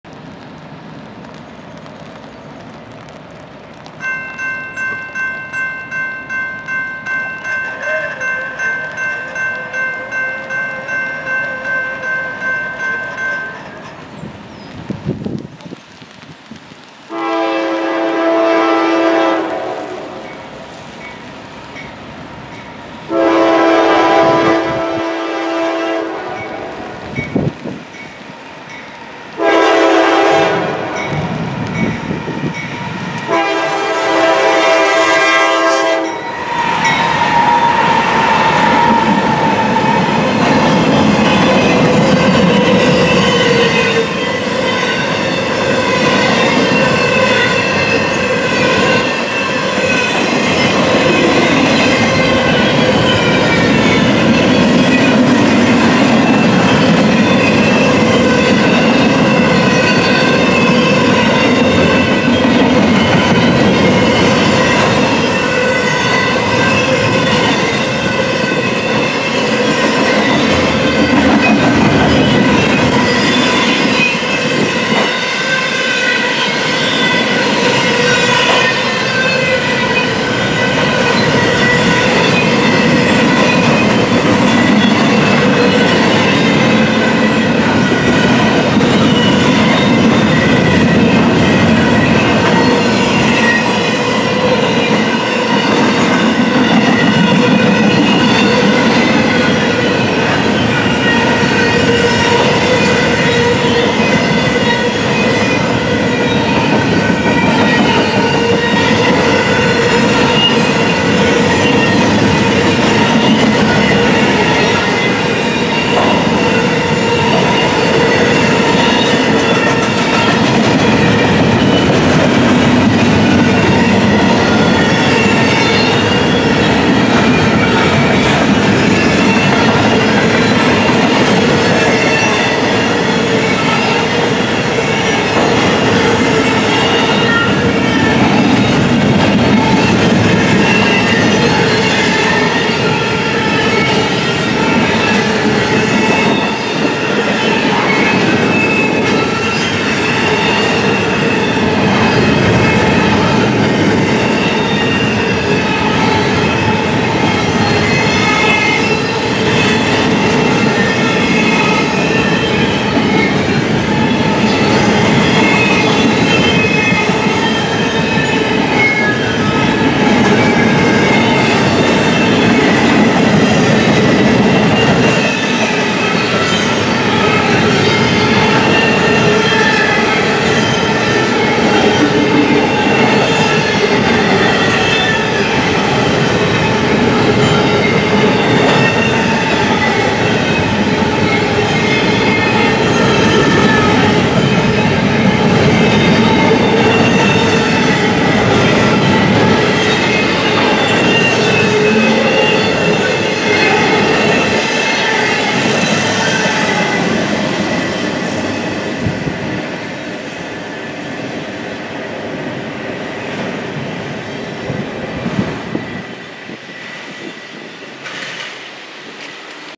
NORFOLK SOUTHERN GE AC44C6M Northbound Intermodal Container Train
A northbound Intermodal Container Lead by Norfolk Southern GE AC44C6M #4406 Engine passing through at Milepost Rosemont.